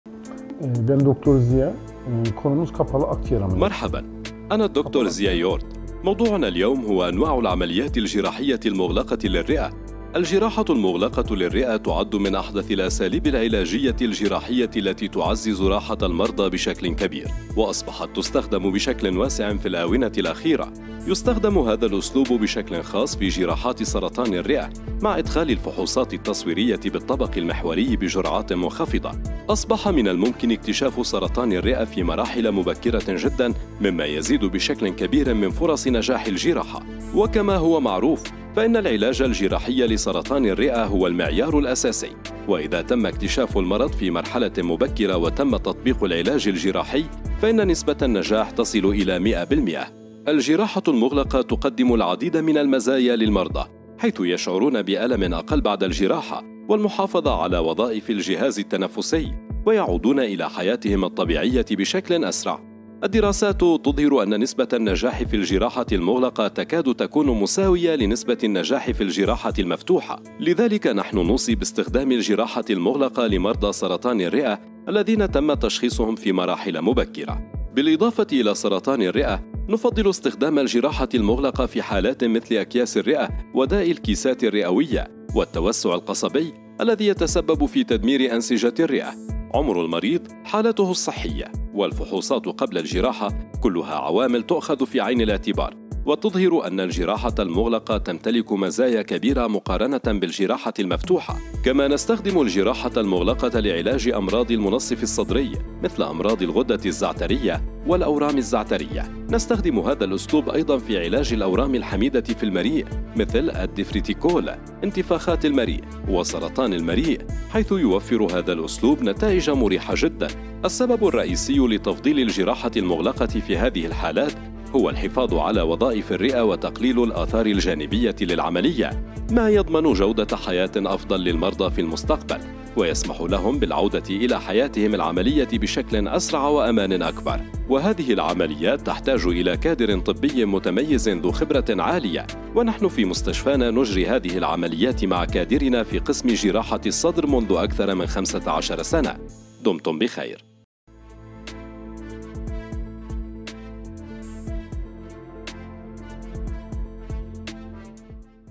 The Future of Lung Surgery is here
Join Dr. Ziya Yurt as he explores the revolutionary world of closed lung surgeries using the video thoracoscopic technique, a pinnacle of surgical excellence in the field of minimally invasive surgery. This innovative approach not only enhances patient recovery rates but also significantly reduces complications associated with traditional lung surgery methods. Discover how healthcare advancements are transforming the treatment of chronic lung conditions, including COPD, lung cancer, and other acute lung diseases.

In this insightful video, Dr. Yurt showcases the benefits of bronchoscopic procedures and interventional pulmonology techniques that are setting new standards in lung treatment. Learn about the symptoms of lung disease, including the alarming signs associated with lung cancer and pneumonia. This video is essential for anyone looking to understand the latest developments in respiratory advancements and chronic lung care. Experience firsthand how these state-of-the-art surgical methods are reshaping the landscape of lung surgery. Don't miss out on this opportunity to dive deep into the future of lung health with expert guidance from Dr. Ziya Yurt.